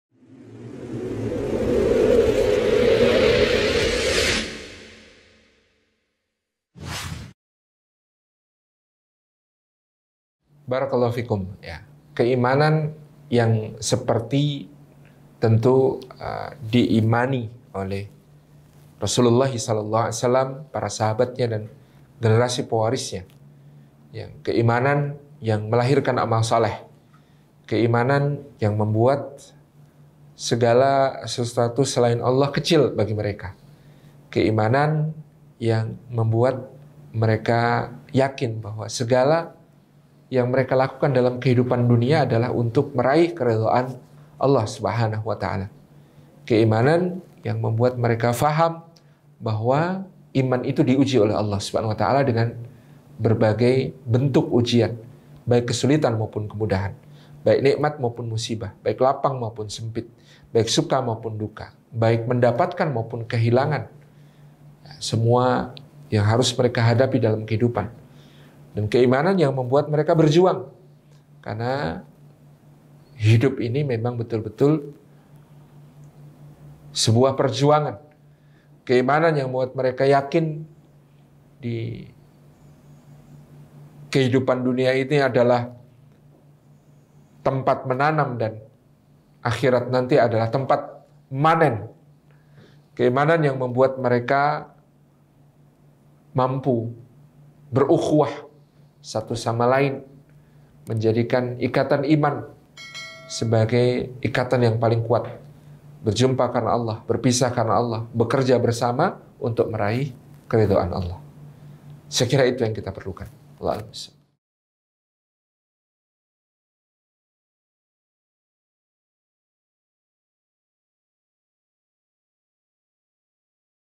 "Iman dan Ukhuwah" | Ust. Salim A. Fillah | Q&A KAJIAN ONLINE HIMPASS
Liputan sesi tanya-jawab Kajian Online Himpass, "Merawat Indonesia dengan Kepemimpinan dan Karya" | Q&A: "Iman dan Ukhuwah" bersama Ustadz @salimafillahofficial pada hari Ahad, 22 Agustus 2021 di Omah Dakwah, Yogyakarta.

Simak kajian lengkapnya di sini:
"Merawat Indonesia dengan Kepemimpinan dan Karya" | Ustadz Salim A. Fillah | KAJIAN ONLINE HIMPASS

Support and follow Pro-You Channel for latest updates!